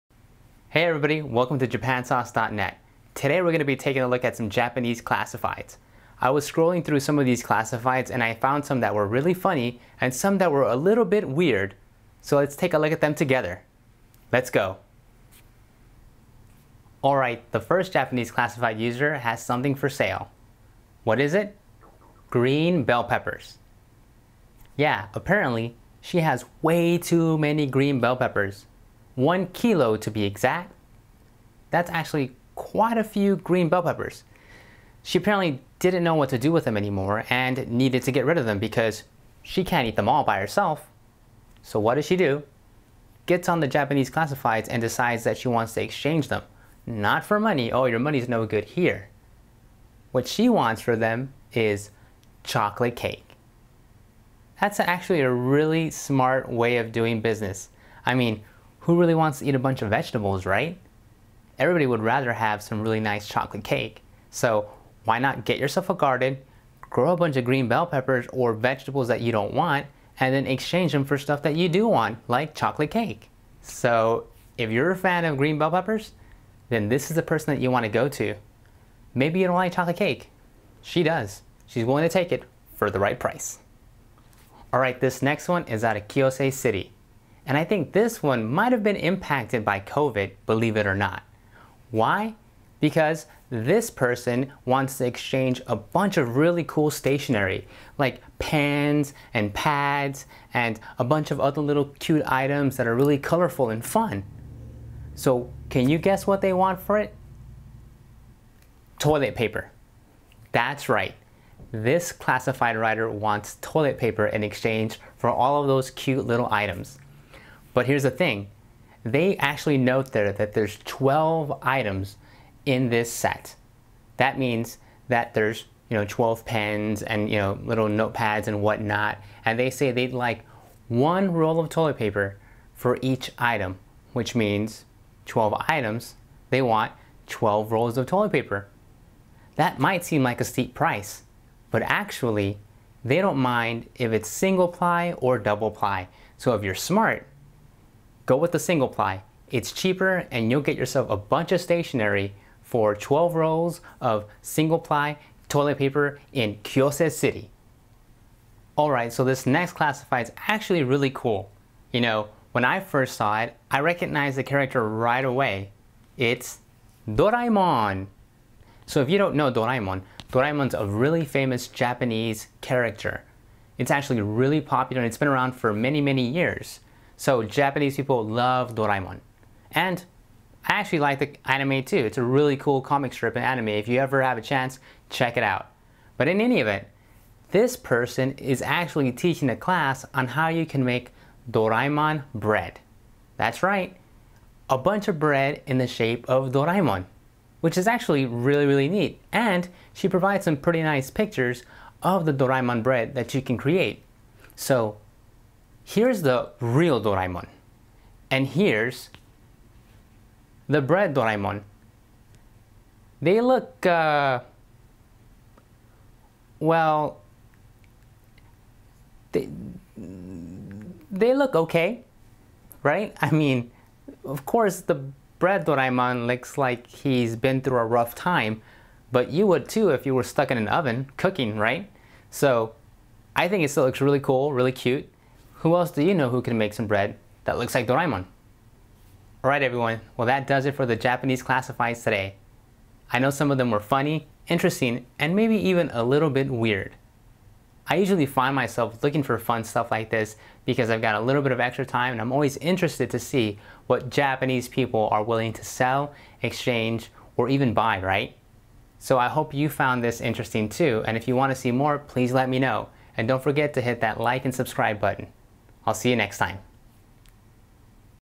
Funny and Weird Japanese Classifieds Ads! All Real!
Today we are taking a look at some funny and weird classified ads from Japanese websites. These are all real!

Hope you enjoy! (And sorry about the glare on my glasses).

Also, if you want to know more about all of the food, fun, and culture of Japan, then checkout

If you want to see more fun videos, then be sure to check out these: